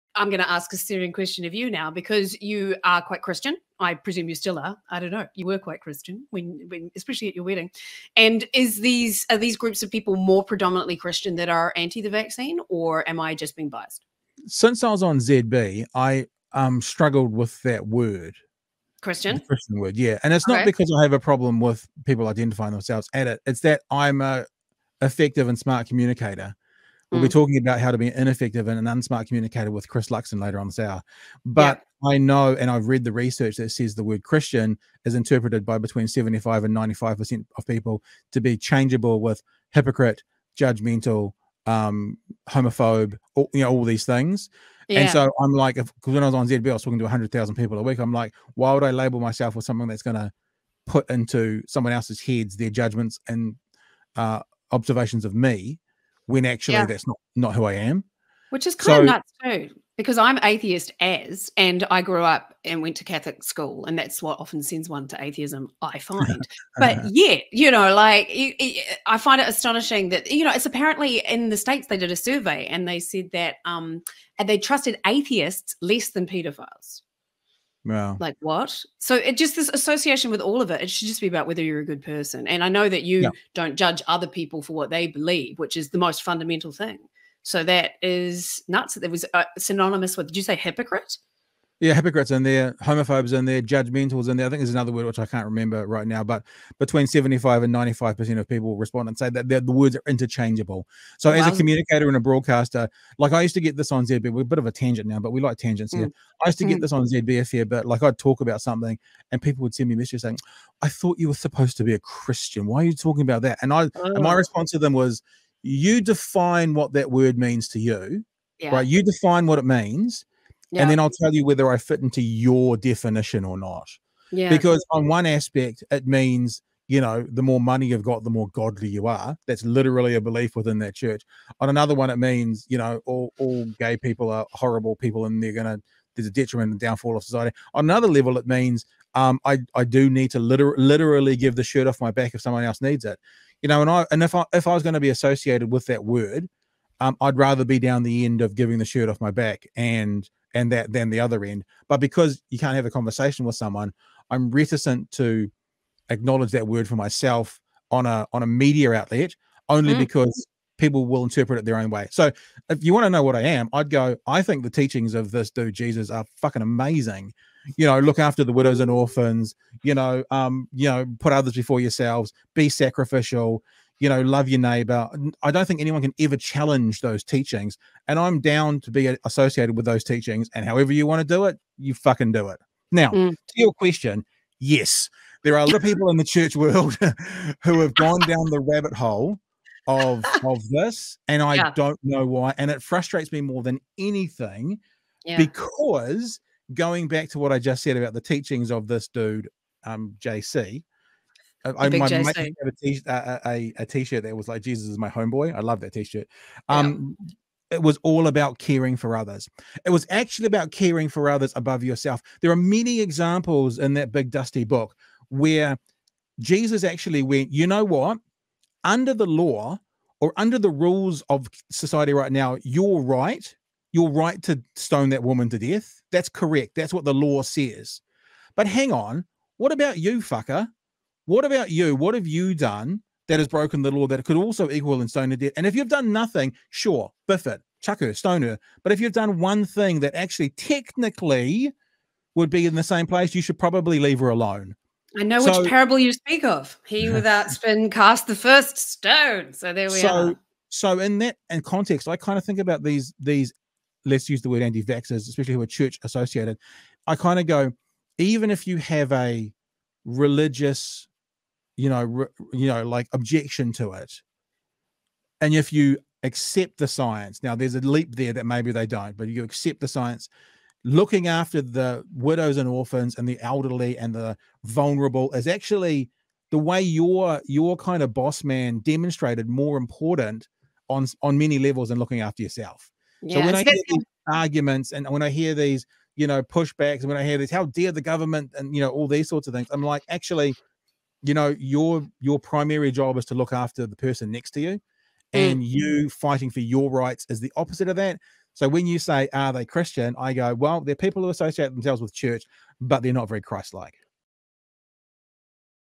BHN Are anti vaxxers Christian, Pat you should know
Penny questions Pat's long association with the church and ponders if Christian's are over represented in the anti-vaxxer movement